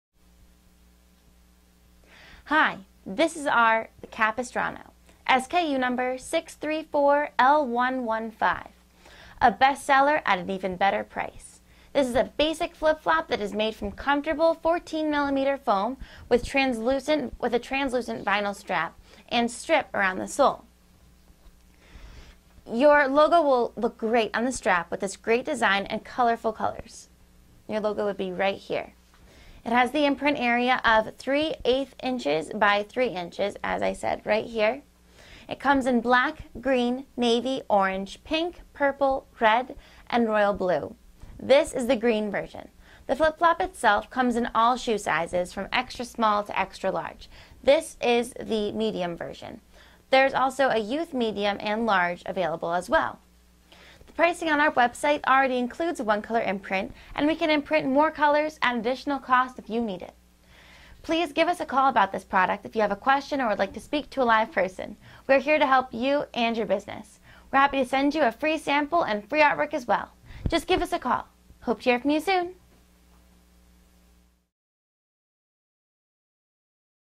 The Capistrano
Our best selling basic flip flop is made of comfortable 14mm foam with vinyl straps, and has a sporty racing strip around the sole. (Note: all straps are translucent except for black.)  A one color imprint on the outside strap of both shoes is included in the price.  (Sole cannot be imprinted)  One additional imprint on the inside of the strap of both shoes may be added for orders of 100 or more for an extra cost.  Please call for more details. SKU: 634-L115